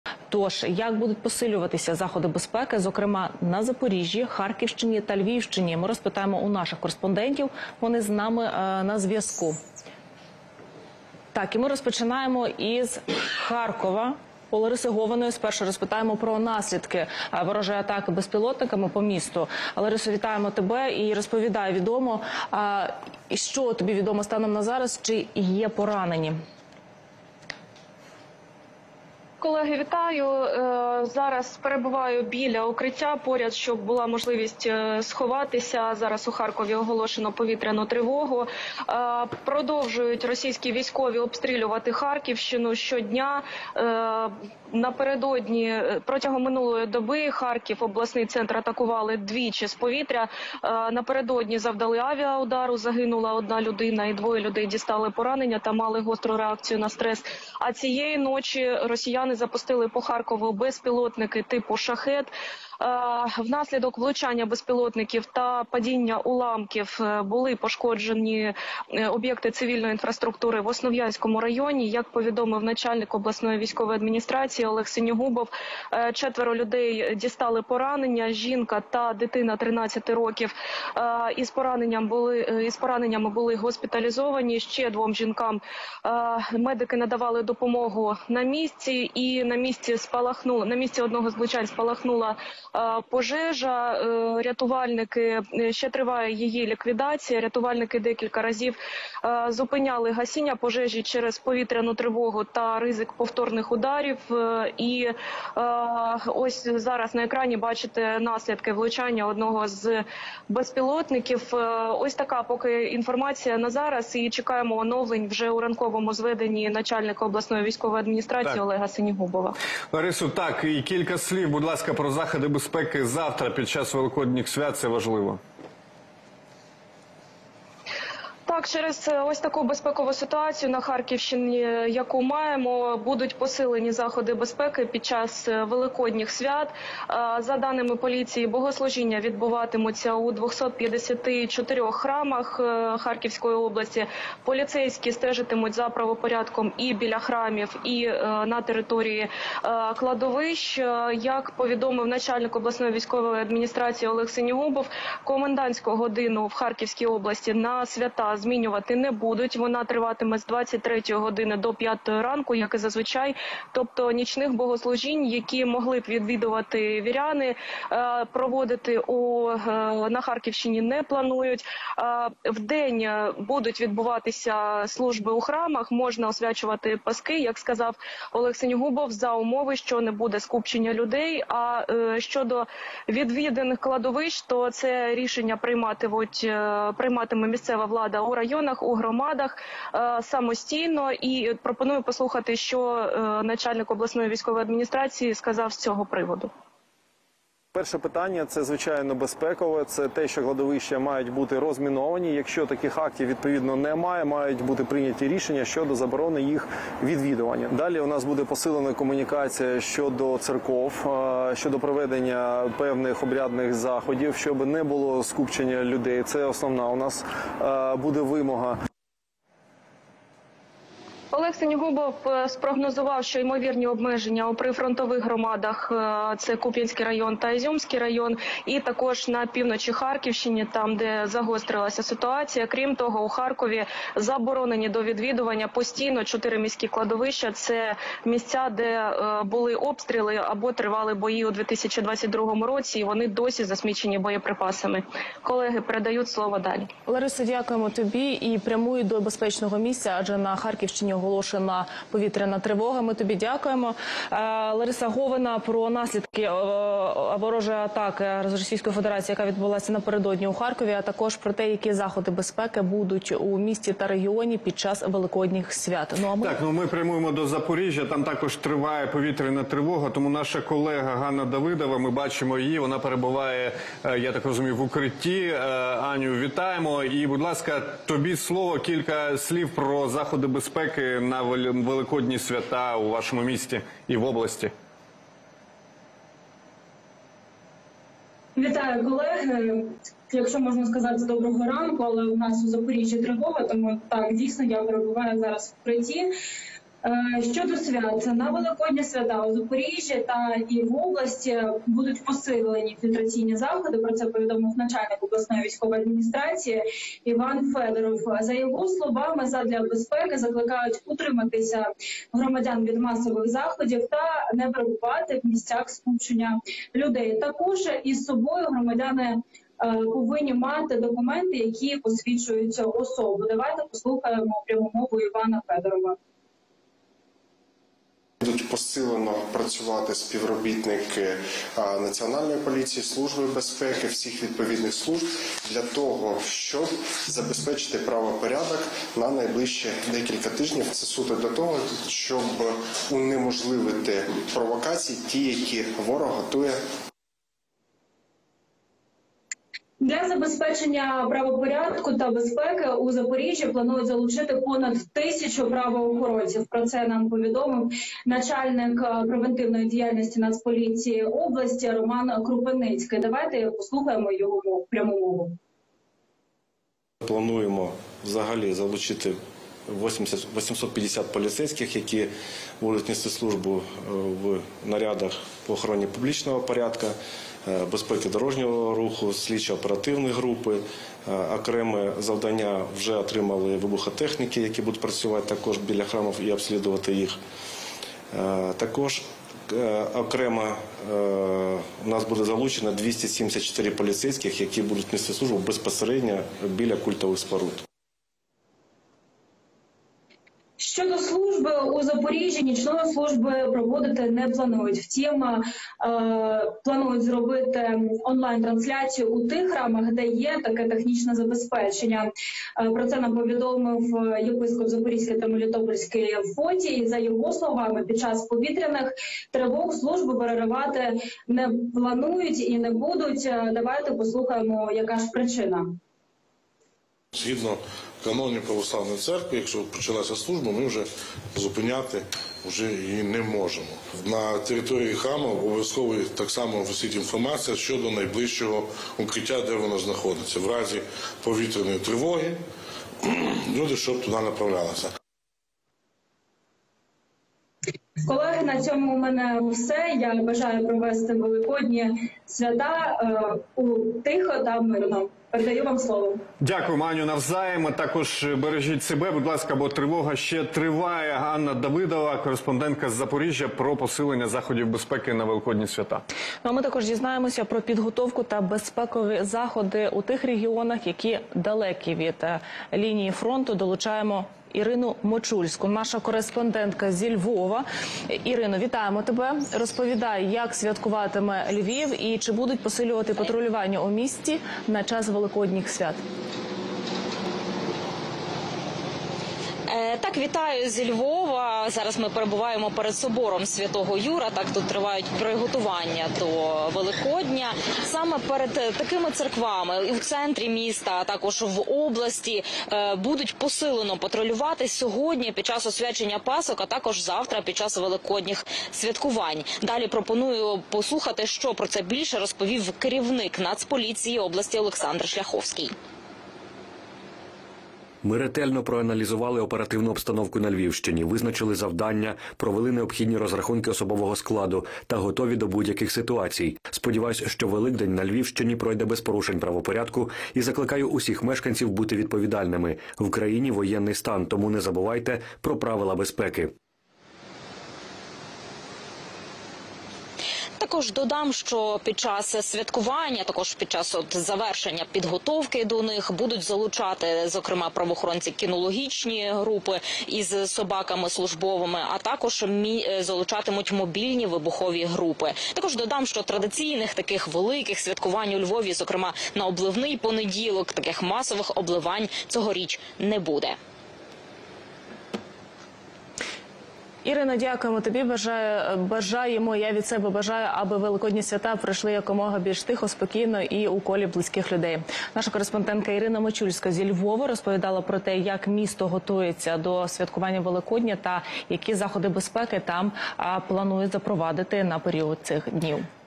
ПЕКЕЛЬНА НІЧ у Харкові! Україна ГОТУЄТЬСЯ ДО СТРАШНІШОГО! Як ПОСИЛЮЮТЬ безпеку під час Великодня?
Ворог вкотре атакував Харків та область шахедами! Які наслідки атаки? Чи є постраждалі? Та які заходи безпеки під час Великодніх свят впроваджують українські міста? Детальніше в сюжеті.